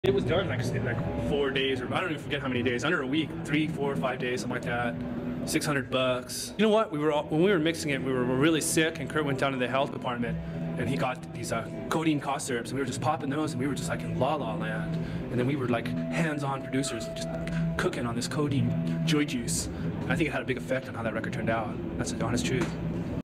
kristnovoselic Snitching On kurtcobain 😂
Krist Novoselic was the star of this Much Music interview with Nirvana in 1991. He goes into a story about the band mixing a record while popping codine cough medicine, and how that medicine influenced the vibe of the album.